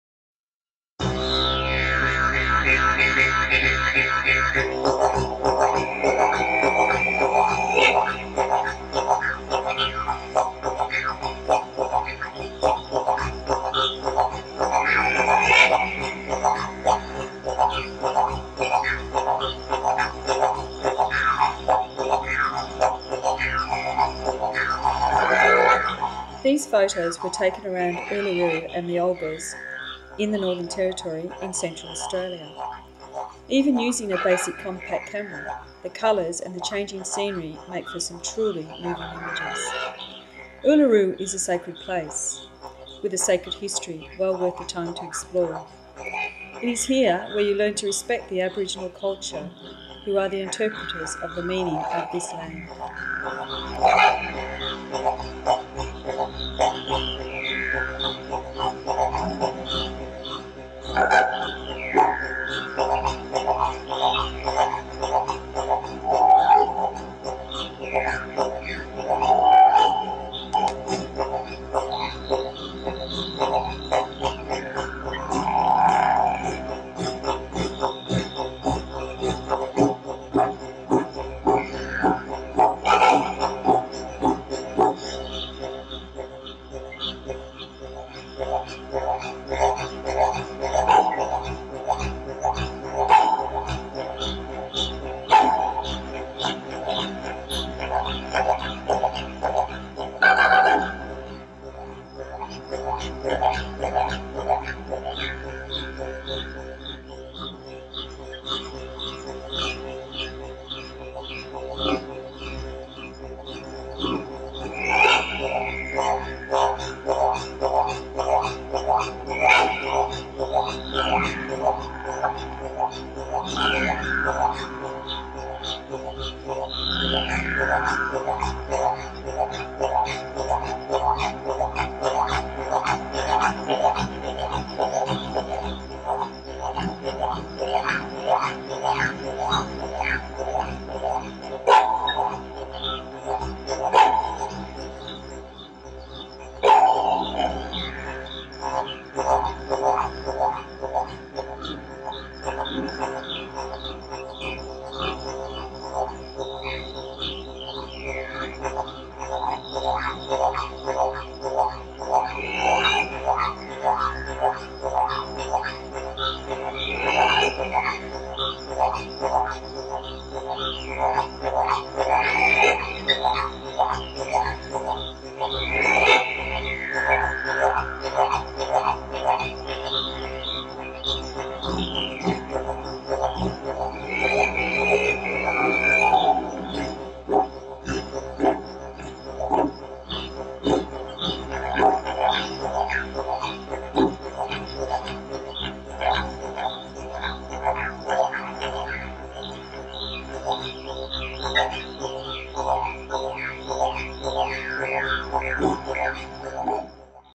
Uluru and the Olgas NT - CCCAVGp
An audio visual presentation capturing some of the colours and aboriginal culture of Ayres Rock (Uluru) and the Olgas (Kata Tjuta). Photography, narration and production by Donna Killeen.